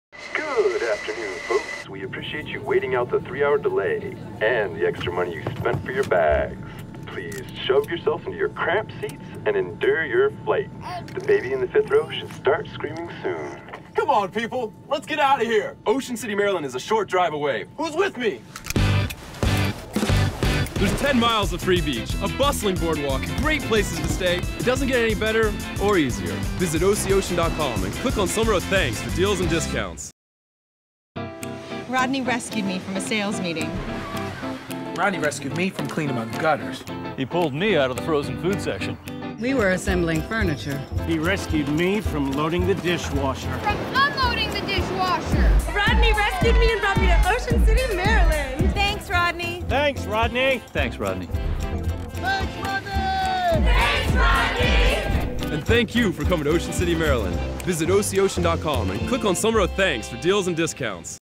Ocean City, Maryland Department of Tourism - "Summer of Thanks" TV commercial
Rodney the OC spokesguard is back in two new spots: the first, where he rescues travelers from the dreaded air travel; and the second, where Rodney's rescuees share their gratitude for their knight in tanned armor.